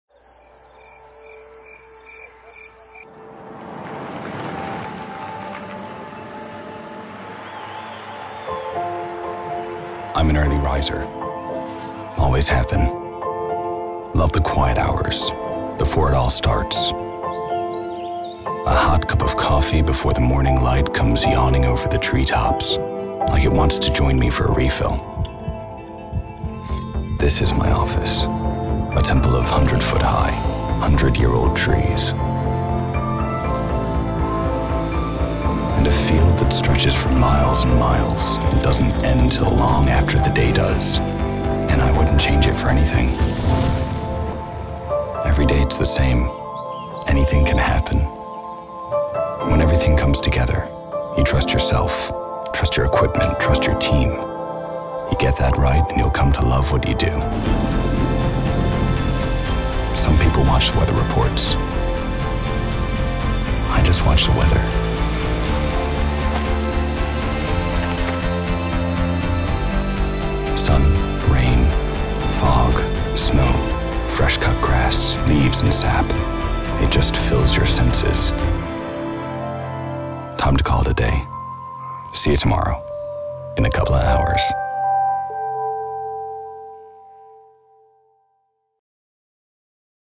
Husqvarna 2016 – Ready when you are. (new)
At Husqvarna we’re proud of being part of your workday.
We provide the equipment, and you get the job done.
So, here’s a tribute to all you Husqvarna users around the world.
Want to see it? Ready when you are.